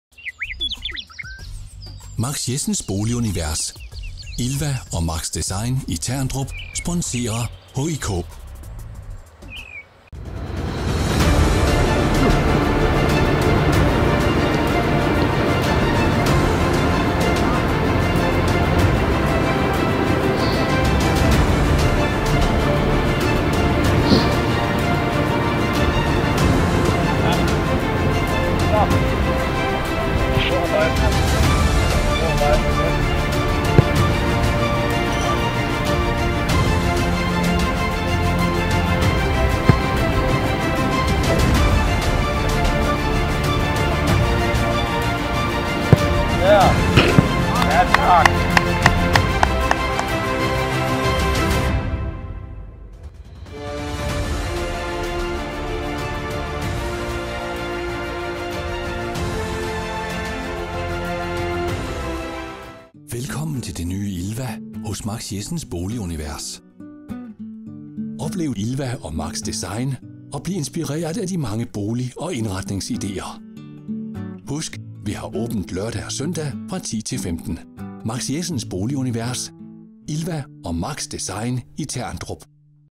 Belek Challenge 2020: Pål Kirkevold
Pål Kirkevold er sidste mand, der skal igennem 'Belek Challenge 2020'. Slår nordmanden mon Oliver Thychosens tid på 29,02 sekunder? ⚽️🏃‍♂️💨